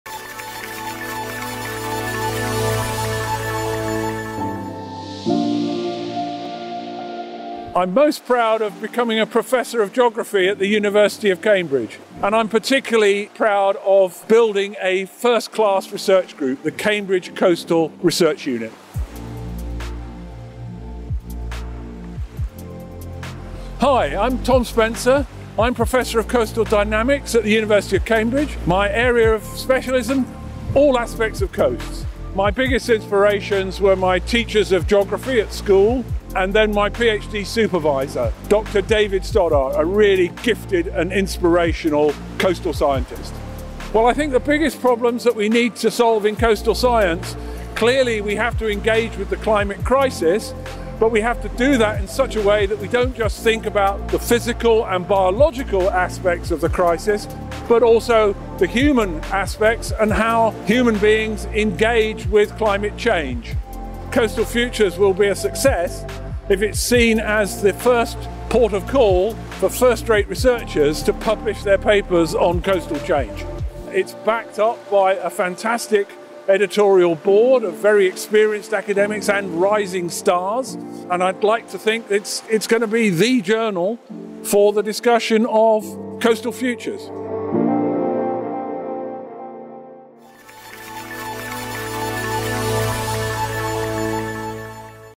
Meet Professor Tom Spencer: Editor-in-Chief of Cambridge Prisms: Coastal Futures
Professor Tom Spencer: A modern-day hero who has dedicated his life’s work to addressing major real-world challenges in the area of coastal futures. Explore Cambridge Prisms: Coastal Futures